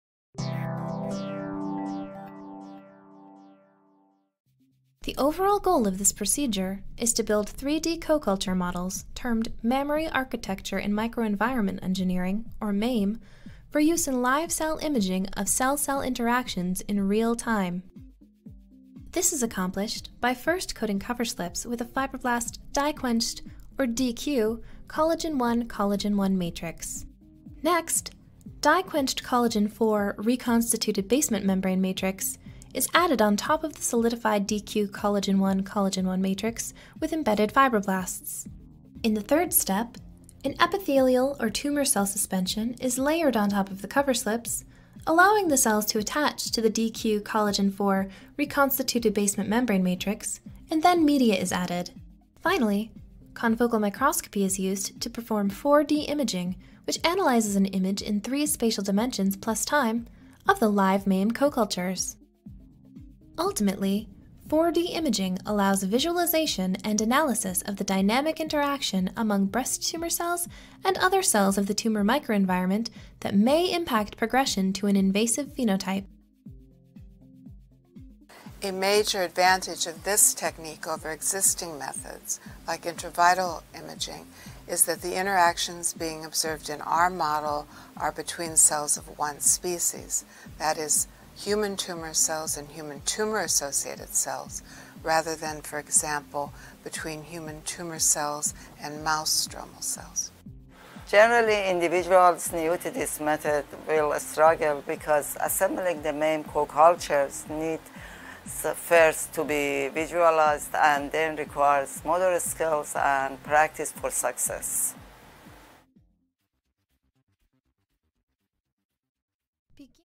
MAME Models For 4D Live-Cell Imaging-Tumor: Microenvironment Interactions l Protocol Preview
MAME Models for 4D Live-cell Imaging of Tumor: Microenvironment Interactions that Impact Malignant Progression -  a 2 minute Preview of the Experimental Protocol

Mansoureh Sameni, Arulselvi Anbalagan, Mary B. Olive, Kamiar Moin, Raymond R. Mattingly, Bonnie F. Sloane
Wayne State University , Department of Pharmacology; Wayne State University , Barbara Ann Karmanos Cancer Institute;

We have developed 3D coculture models for live-cell imaging in real-time of interactions among breast tumor cells and other cells in their microenvironment that impact progression to an invasive phenotype.  These models can serve as preclinical screens for drugs to target paracrine-induced proteolytic, chemokine/cytokine and kinase pathways implicated in invasiveness.